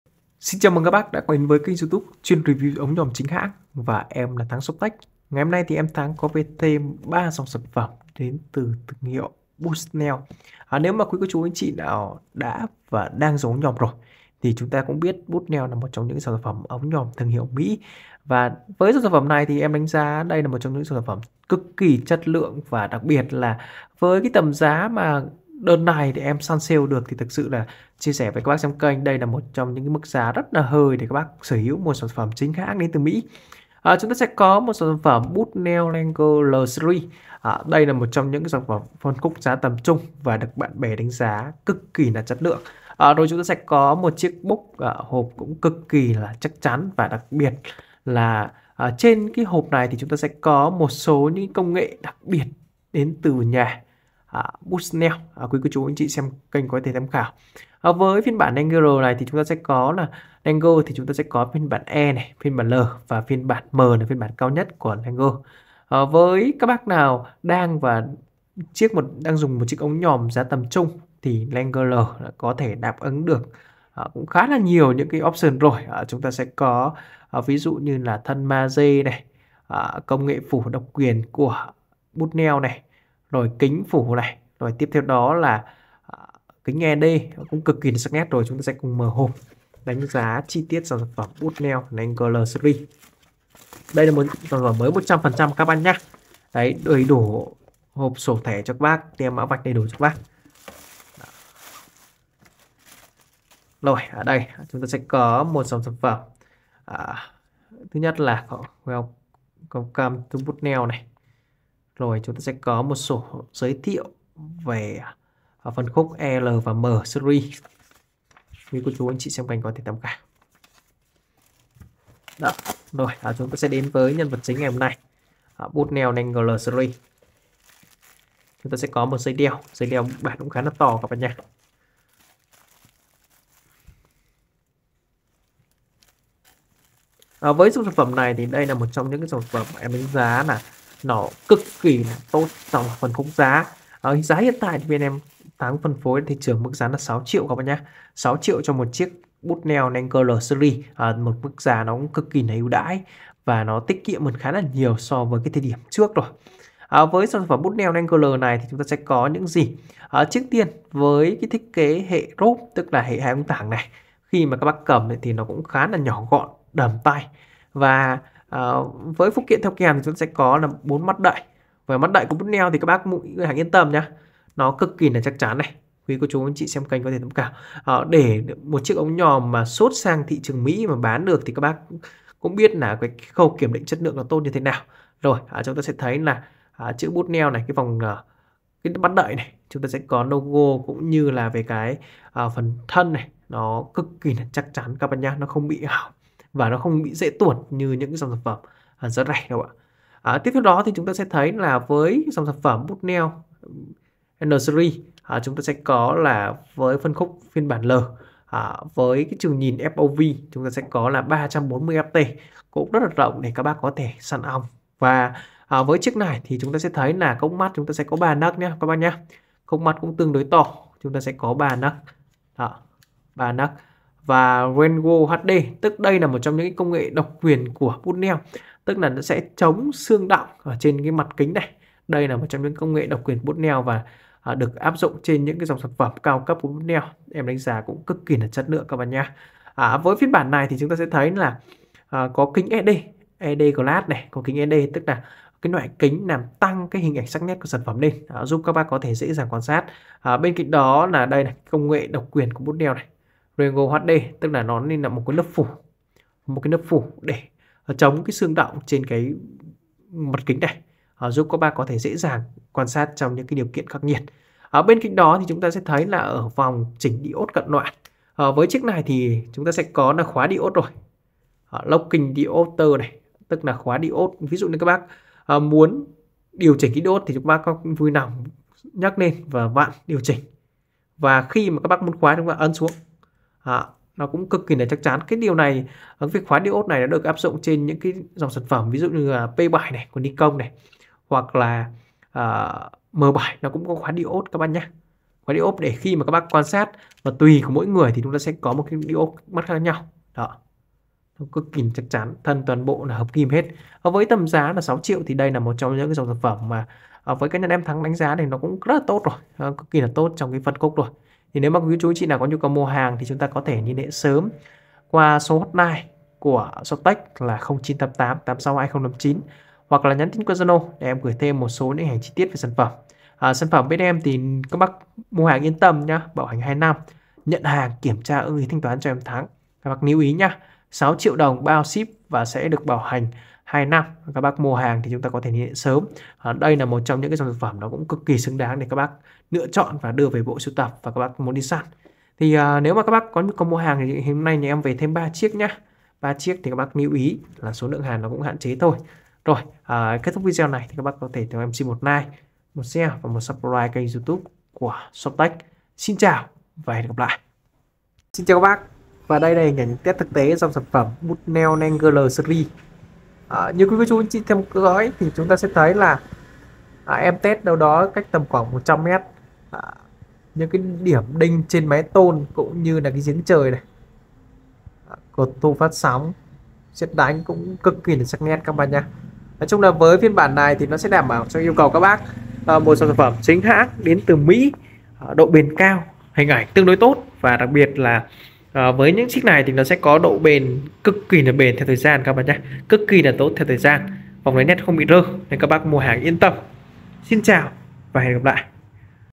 Ống nhòm Bushnell Lengend L chuyên dụng săn ong du lịch, được đánh giá tốt nhất phân khúc 200 đô
♦️♦️♦️ Địa chỉ: Số 3 hẻm 140/1/2 ngõ 140 Nguyễn Xiển - Hà Nội